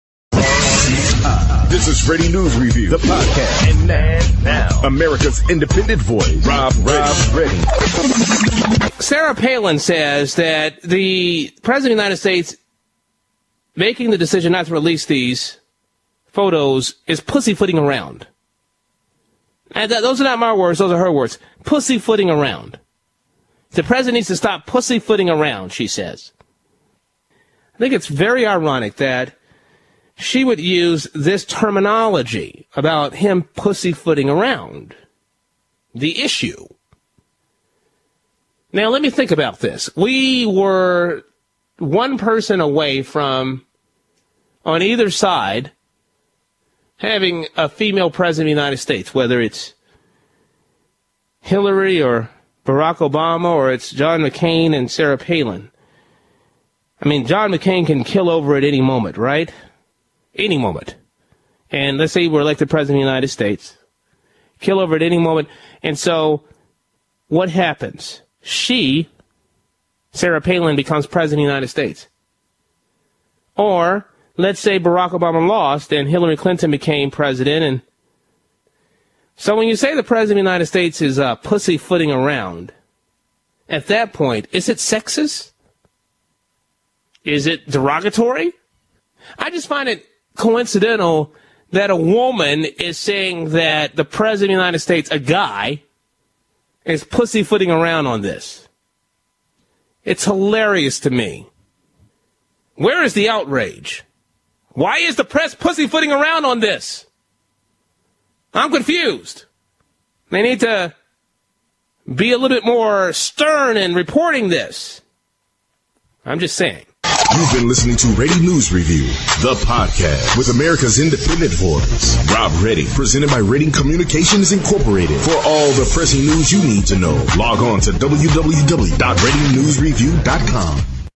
Rob Redding rips Sarah Palin for bin Laden 'pussyfooting' comment
I do not understand why a woman who wanted to be second in line to be president would use such language.